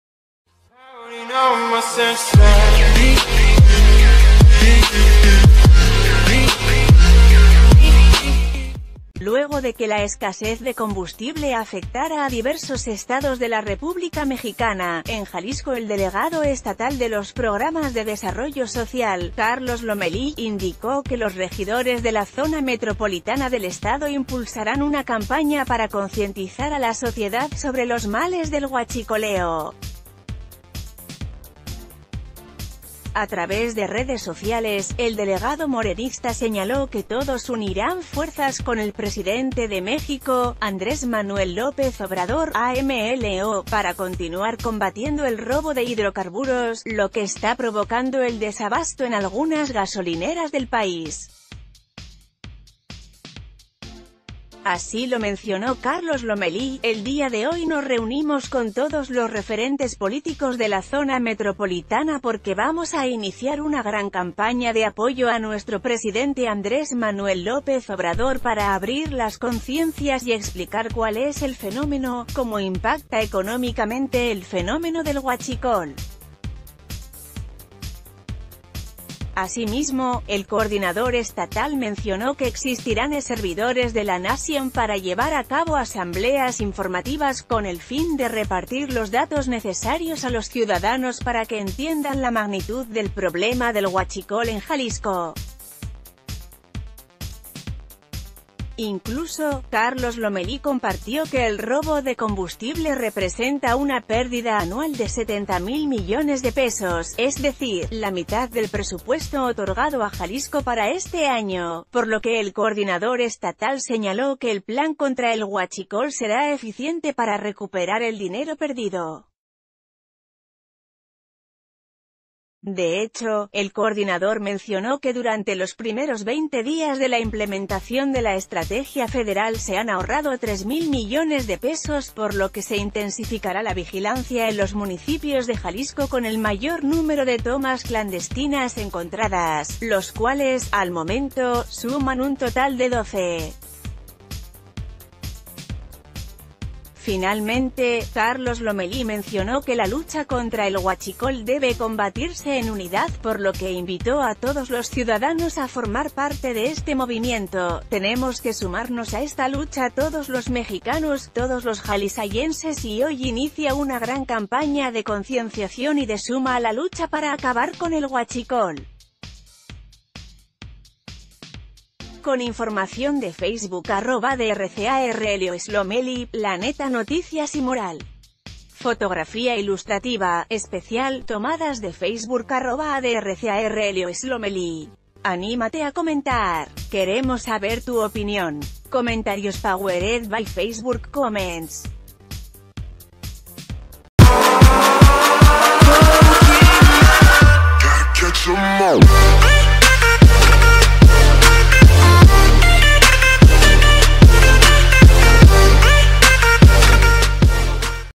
Carlos Lomelí anuncia campaña de concientización sobre huachicoleo en Jalisco
Luego de que la escasez de combustible afectara a diversos estados de la república mexicana, en Jalisco el delegado estatal de los programas de Desarrollo Social, Carlos Lomelí, indicó que los regidores de la zona metropolitana del estado impulsarán una campaña para concientizar a la sociedad sobre ...